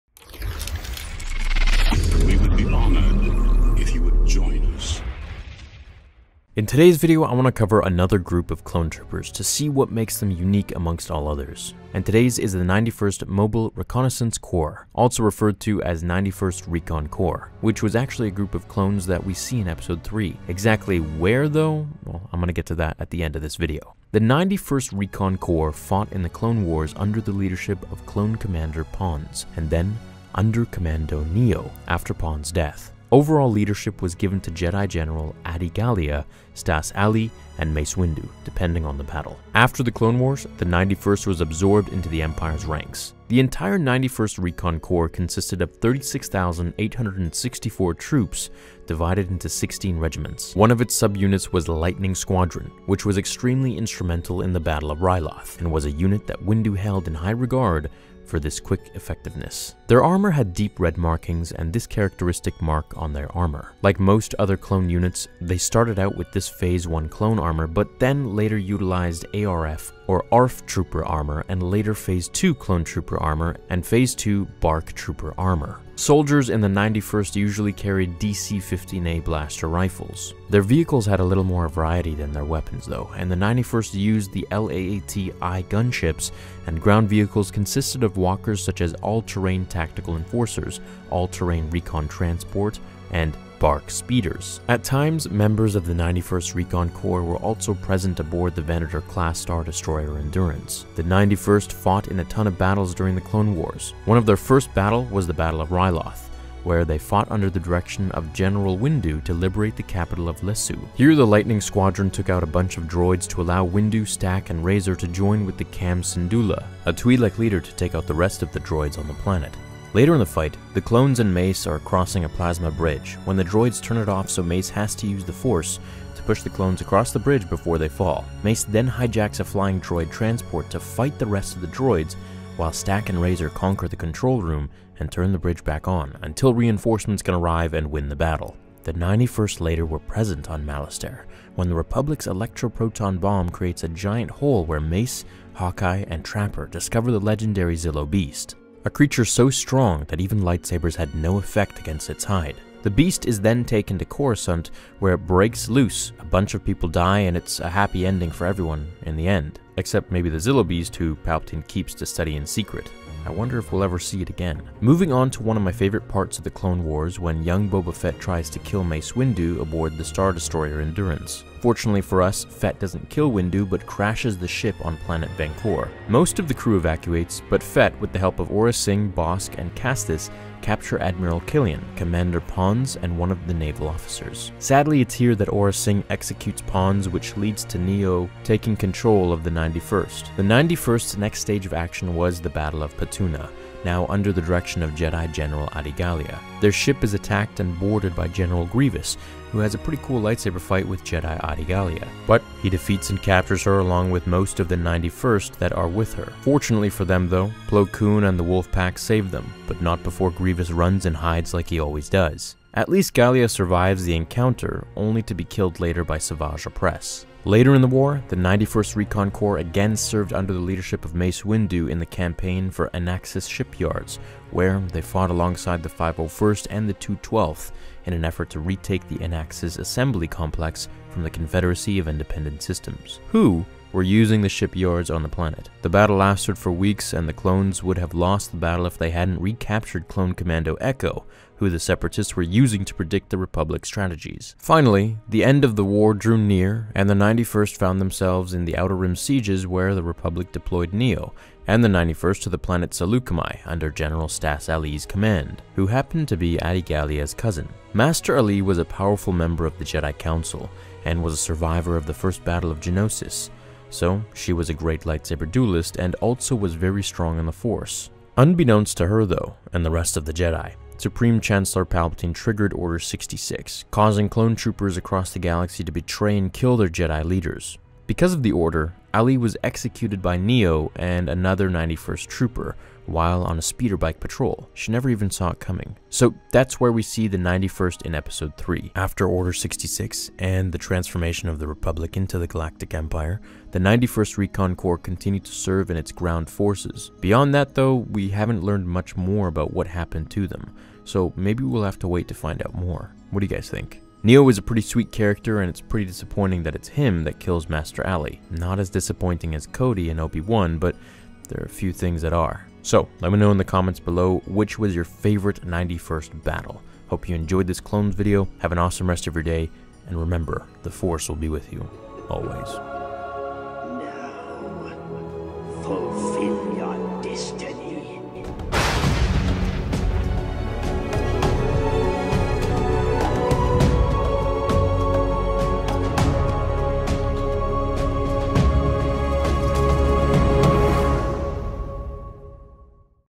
Who are These Clone Troopers from Order 66? - Clone Wars Explained
The 91st Recon Corps were awesome in the Clone Wars, and will be showing up in Season 7 a lot more. They were also seen in Revenge of the Sith during Order 66. Here's what you need to know about these Clones!